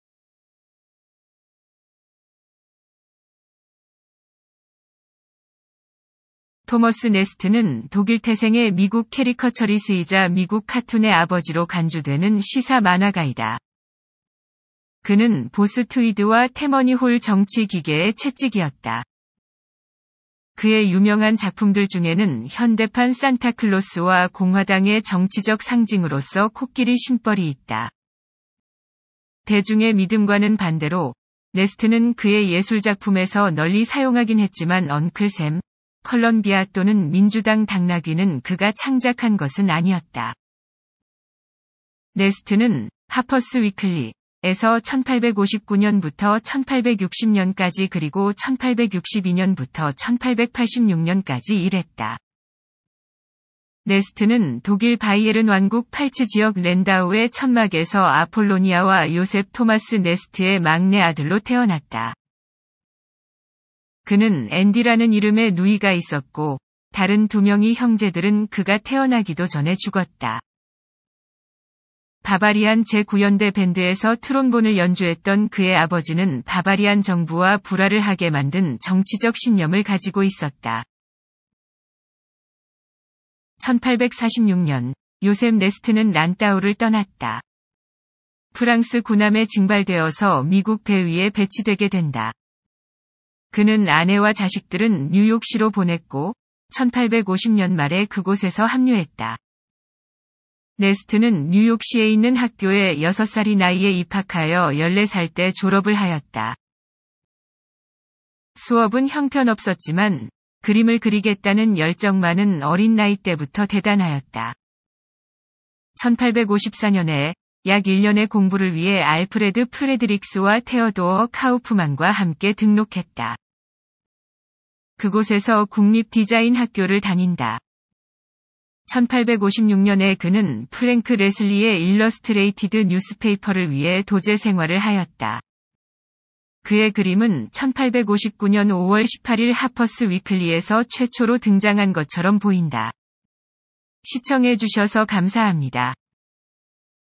토머스 내스트
토머스 내스트（1840년 9월 27일 – 1902년 12월 7일）는 독일 태생의 미국 캐리커쳐리스이자 미국 카툰의 아버지로 간주되는 시사 만화가이다.
그는 보스 트위드와 태머니 홀 정치 기계의 채찍이었다.
그의 유명한 작품들 중에는 현대판 산타클로스와 공화당의 정치적 상징으로서 코끼리 심벌이 있다.
대중의 믿음과는 반대로, 내스트는 그의 예술작품에서 널리 사용하긴 했지만 엉클 샘（미국 사람을 상징하는 남성 의인화）, 컬럼비아（미국적 가치를 나타낸 여성 의인화） 또는 민주당 당나귀는 그가 창작한 것은 아니었다.
내스트는 「하퍼스 위클리」에서 1859년부터 1860년까지 그리고 1862년부터 1886년까지 일했다.
내스트는 독일 바이에른 왕국 팔츠 지역 랜다우의 천막에서 아폴로니아와 요셉 토마스 내스트의 막내 아들로 태어났다.
그는 앤디라는 이름의 누이가 있었고, 다른 두 명의 형제들은 그가 태어나기도 전에 죽었다.
바바리안 제9연대 밴드에서 트롬본을 연주했던 그의 아버지는 바바리안 정부와 불화를 하게 만든 정치적 신념을 가지고 있었다.
1846년, 요셉 내스트는 란다우를 떠났다.
프랑스 군함에 징발되어서 미국 배 위에 배치되게 된다.
그는 아내와 자식들은 뉴욕 시로 보냈고, 1850년 말에 그곳에서 합류했다.
내스트는 뉴욕 시에 있는 학교에 6살의 나이에 입학하여 14살 때 졸업을 하였다.
수업은 형편없었지만, 그림을 그리겠다는 열정만은 어린 나이 때부터 대단하였다.
1854년에, 약 1년의 공부를 위해 알프레드 프레드릭스와 테어도어 카우프만과 함께 등록했다.
그곳에서 국립 디자인 학교를 다닌다.
1856년에 그는 프랭크 레슬리의 일러스트레이티드 뉴스페이퍼를 위해 도제 생활을 하였다.
그의 그림은 1859년 5월 18일 하퍼스 위클리에서 최초로 등장한 것처럼 보인다.